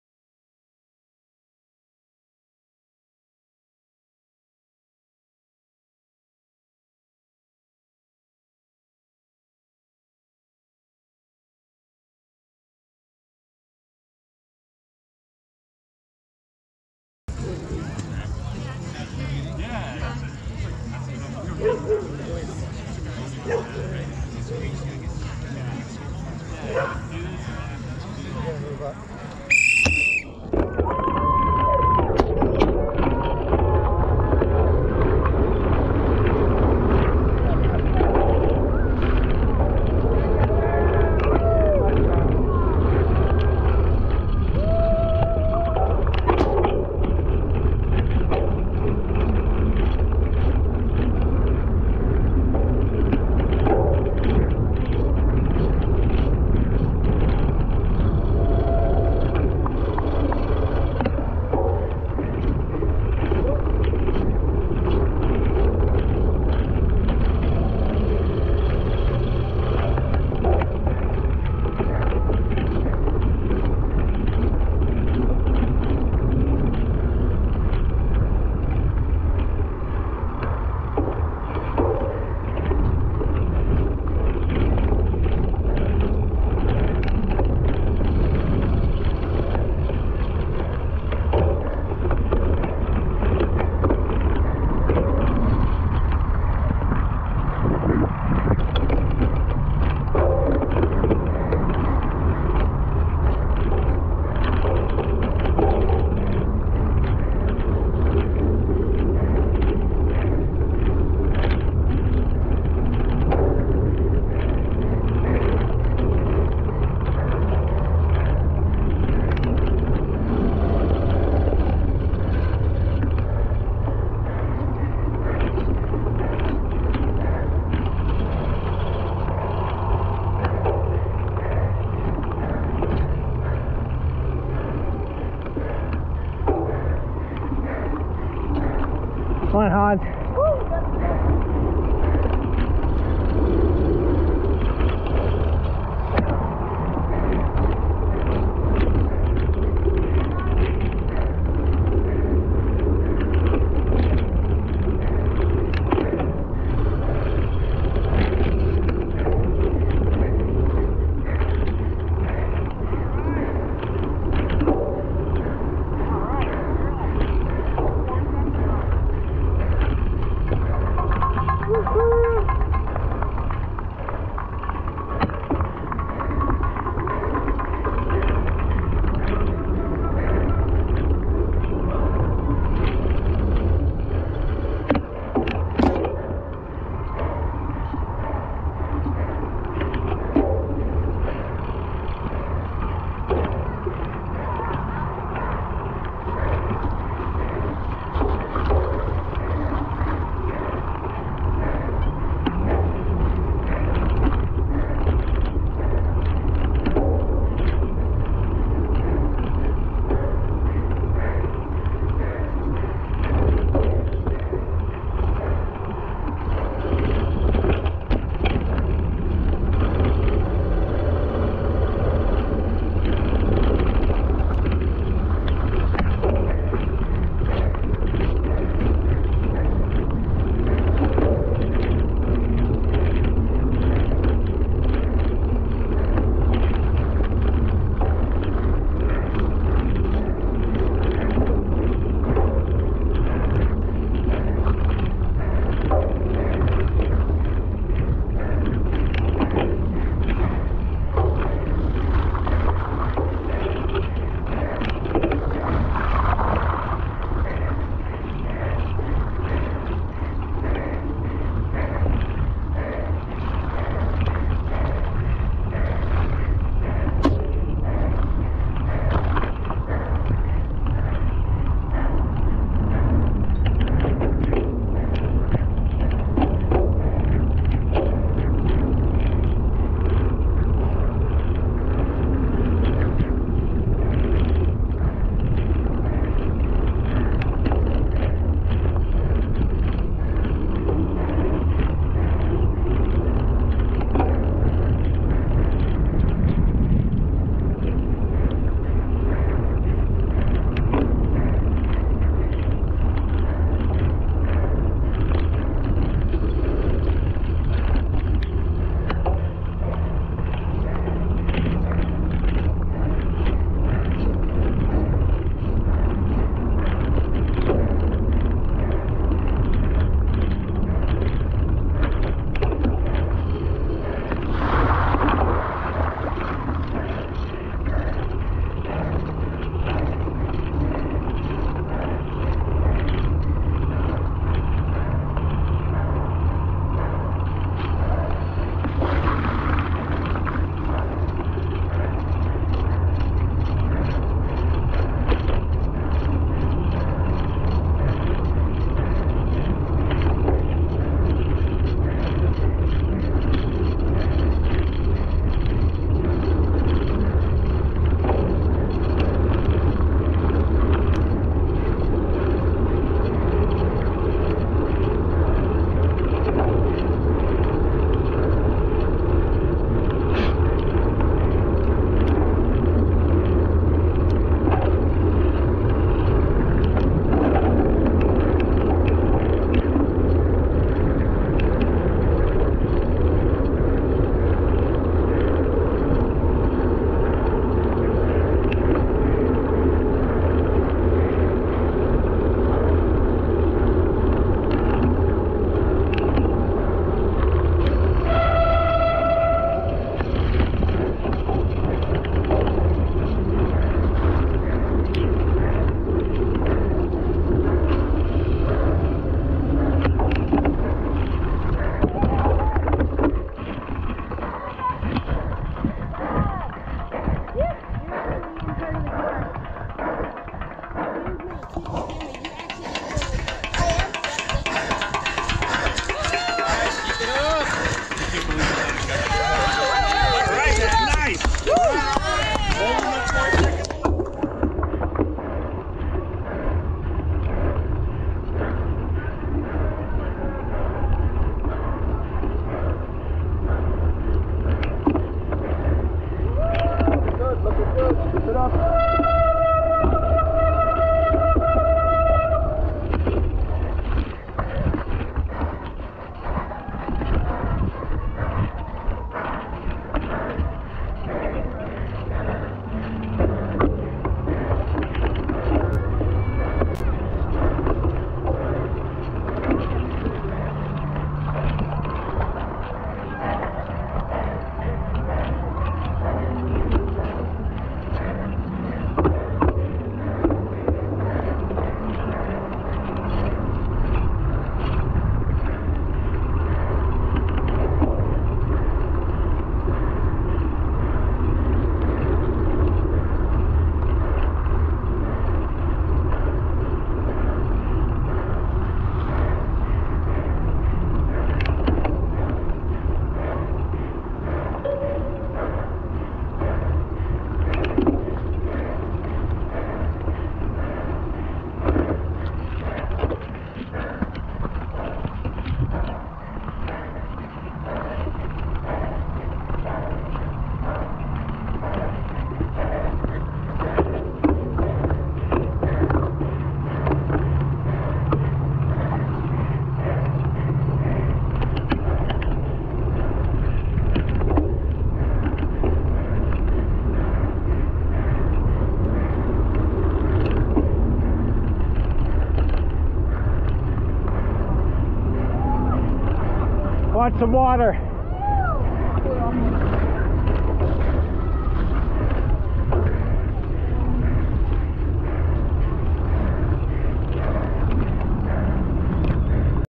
The muddiest 'cross race I've ever done (and I won!)
First lap of the 2021 West Hill Shop (Putney, VT) 35-44 4/5 cx race. So muddy, I love these conditions and it's been a very dry 'cross season in New England. A very small field of just six guys (there was a larger field of 4/5 45+ guys 1 min behind us)--small field meant I got to line up on the front row with Simon and Hans, which was fun. Joel, who came in second, was nipping at my heels this whole first lap and he was making me anxious. I can't tell exactly but I believe I was getting the better of him in the muddiest sections and was able to grow a big lead by 2 laps to go.

I decided to make this event my A race at the beginning of the season and I'm very pleased with how it worked out. I love this venue and shop so much and I knew doing well here and the chance to finish out my long (oh so long) career as a cat 4 on this course would be special. With this win I've now got enough points to start next season as a 3.